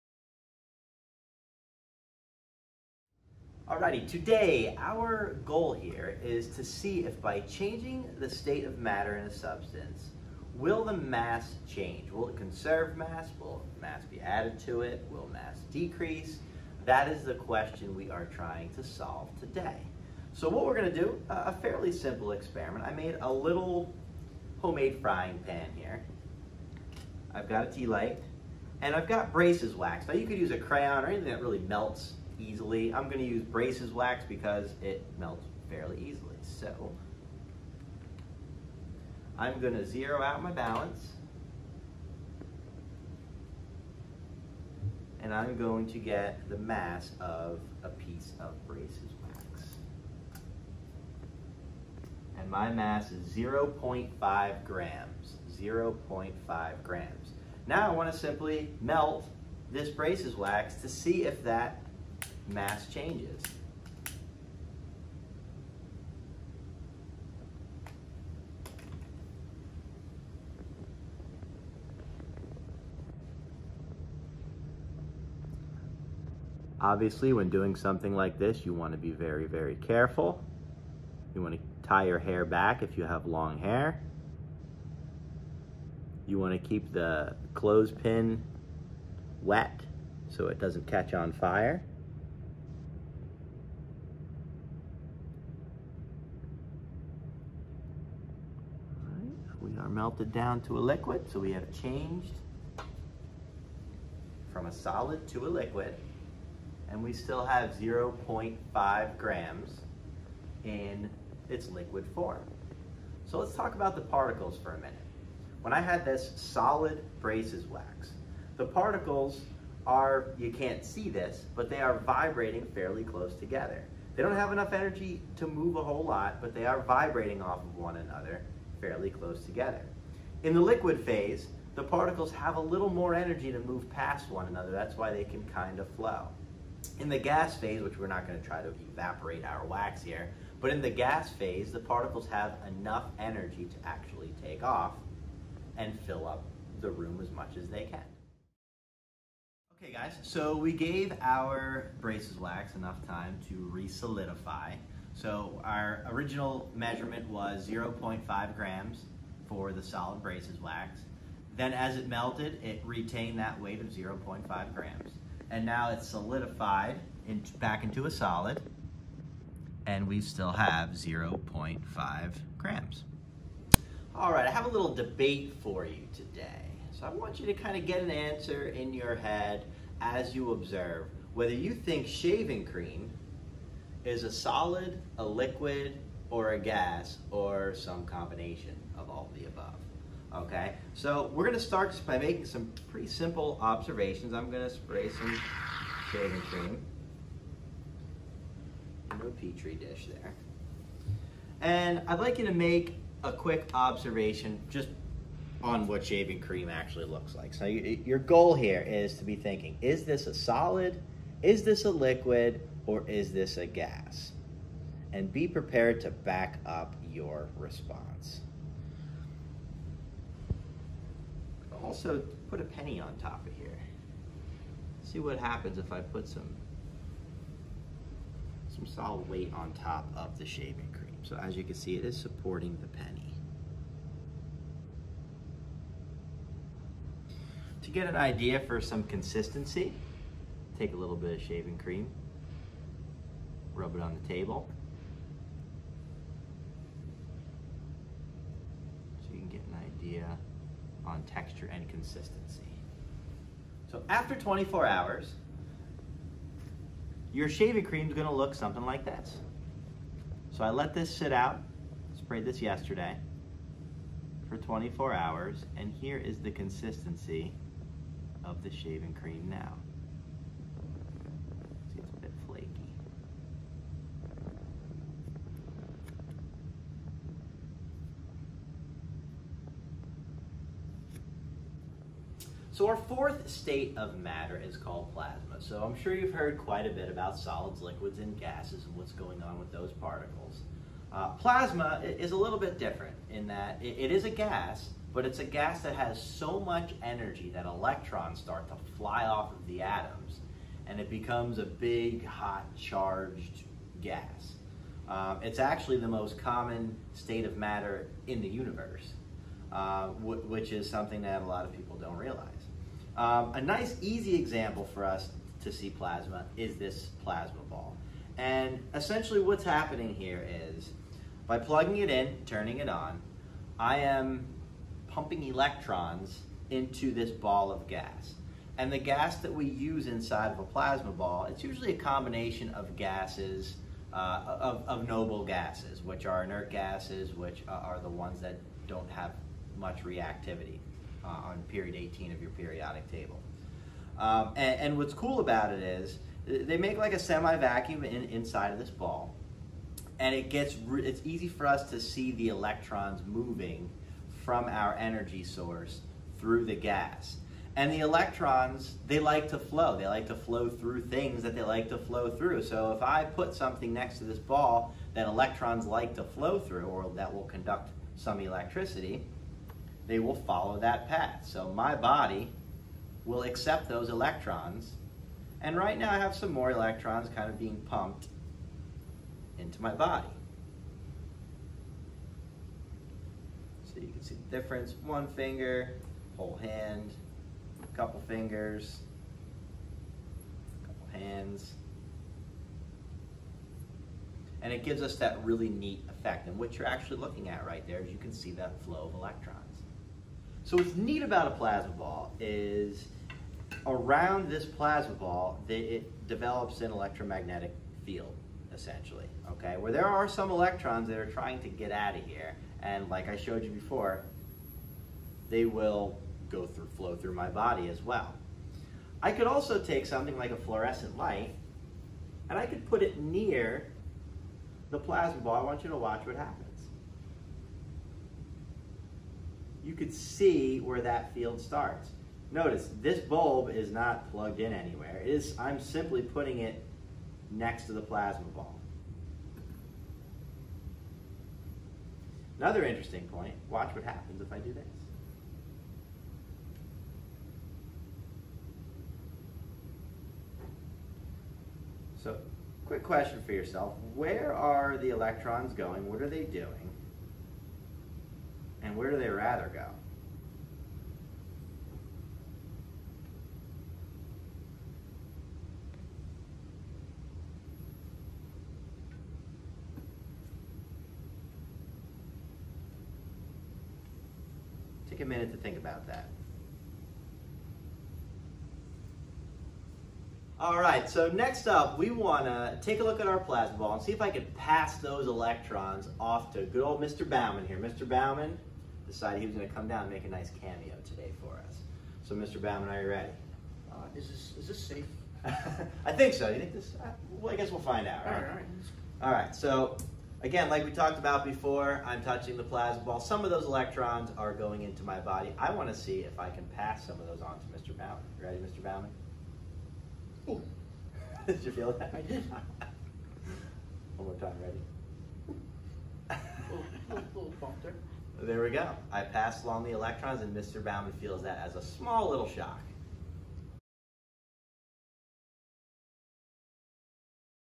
States of Matter Mini Demos
Exploring states of matter! 3 mini demonstrations that you can try at home: conservation of mass, state debate, and plasma ball tricks.